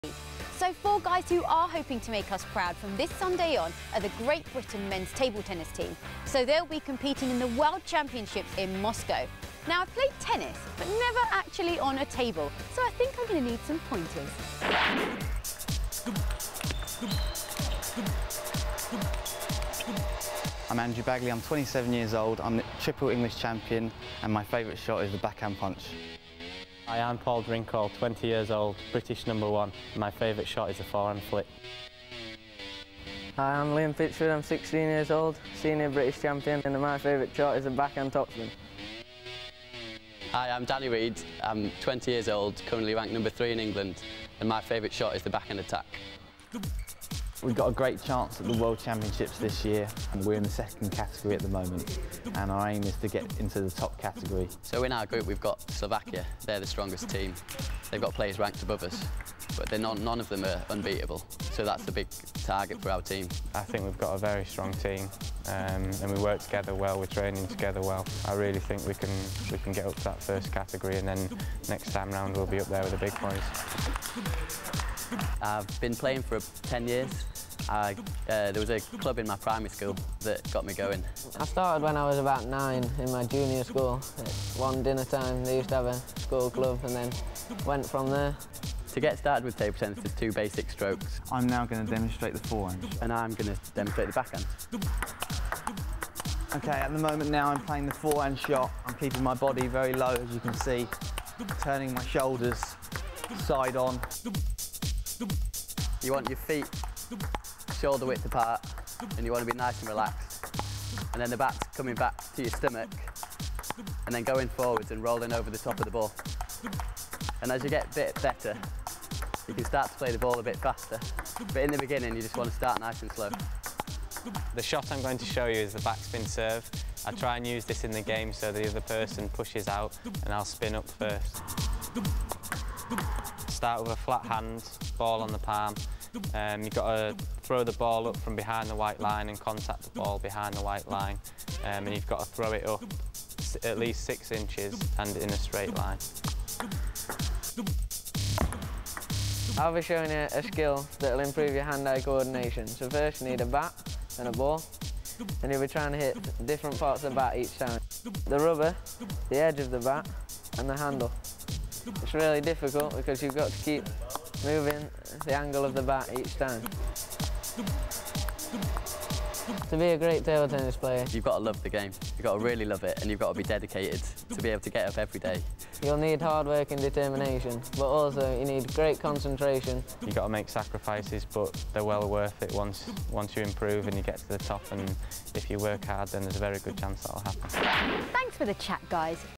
Sports Round World Championships Preview
A clip a about the English Mens Squad just before they packed their bags and travelled to the World Championships in Moscow